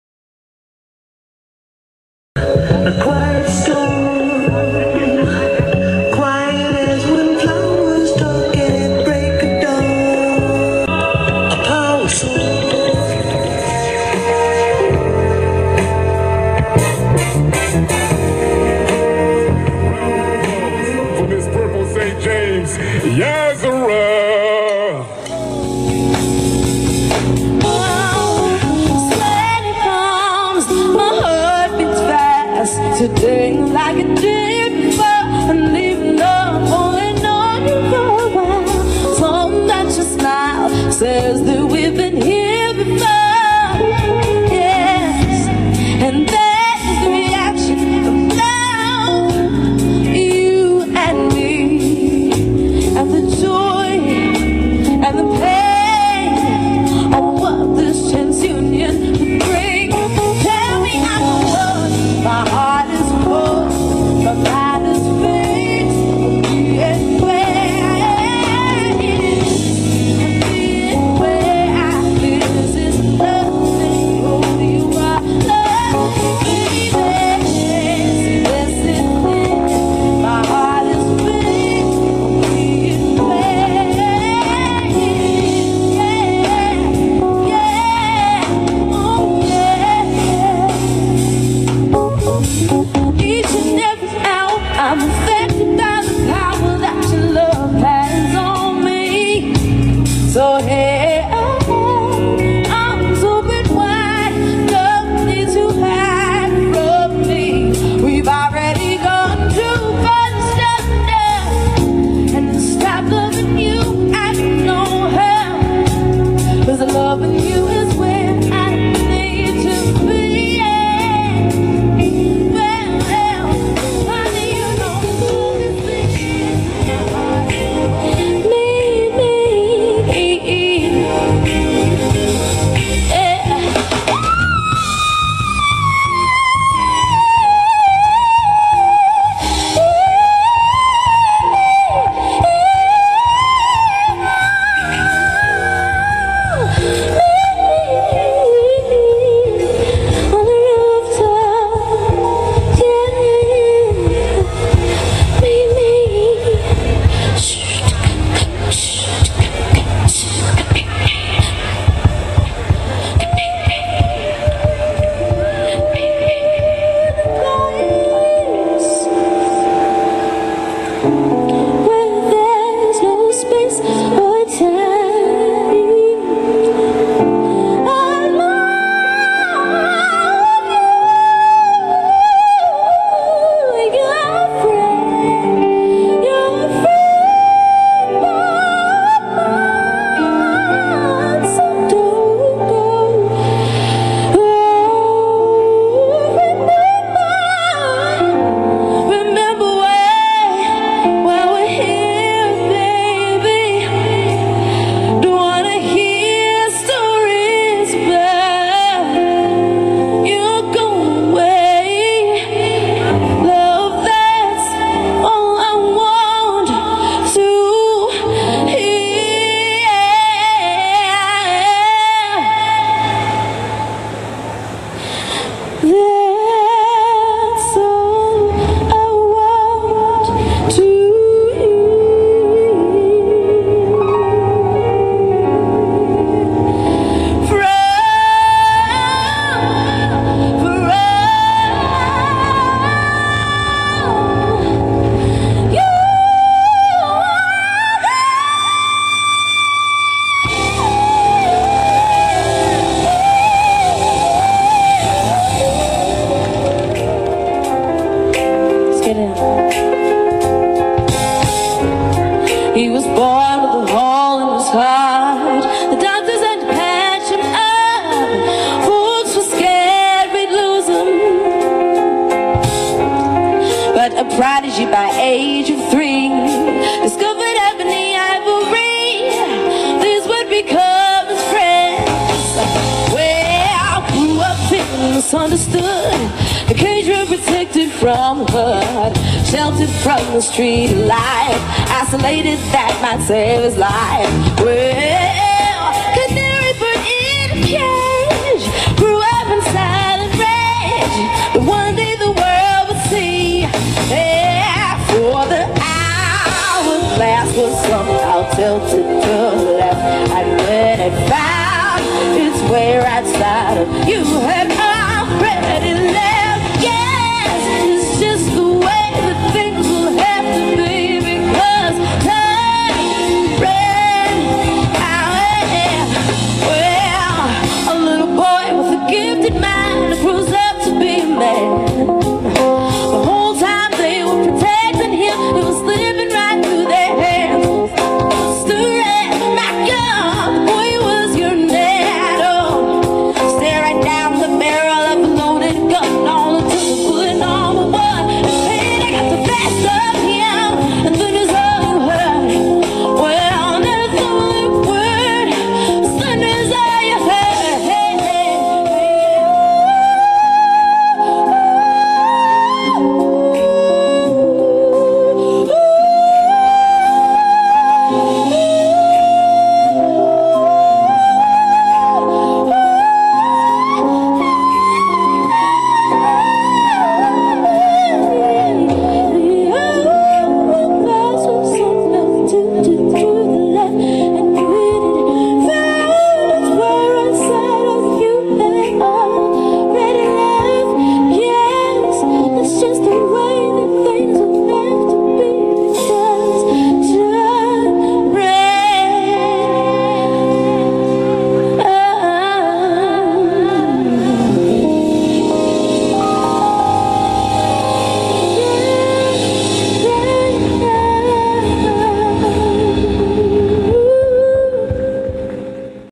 YAHZARAH LIVE at JAZZY SUMMER NIGHTS
On August 6, 2009, Yahzarah aka Purple Saint James brought the heat to the stage at Hopkins Plaza in Baltimore.